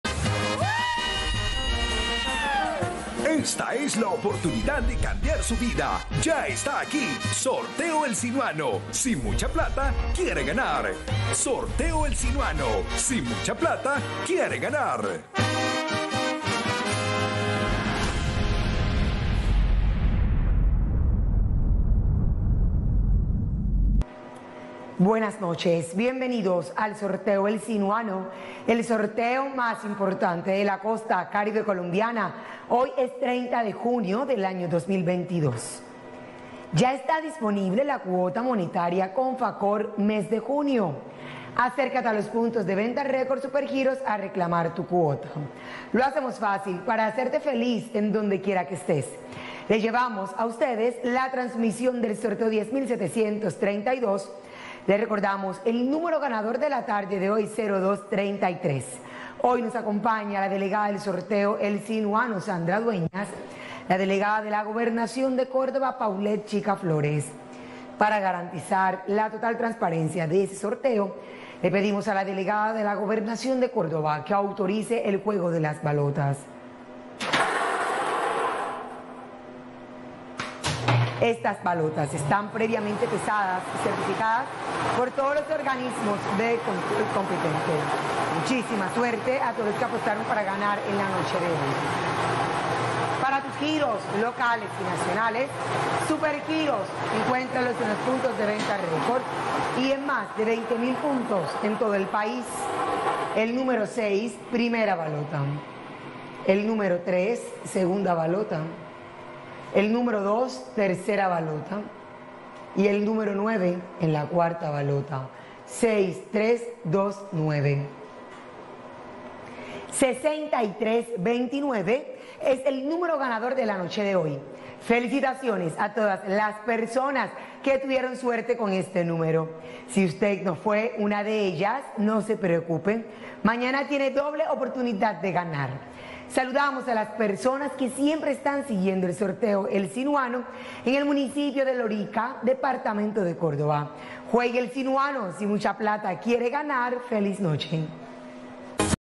EL SINUANO NOCHE Resultados JUEVES 30 DE JUNIO de 2022 ✅🥇🔥💰
Hola buenas noches, te presentamos los resultados de las loterías y chances en Colombia, vamos con el Resultados del Chance Sinuano noche del JUEVES 30 DE JUNIO de 2022.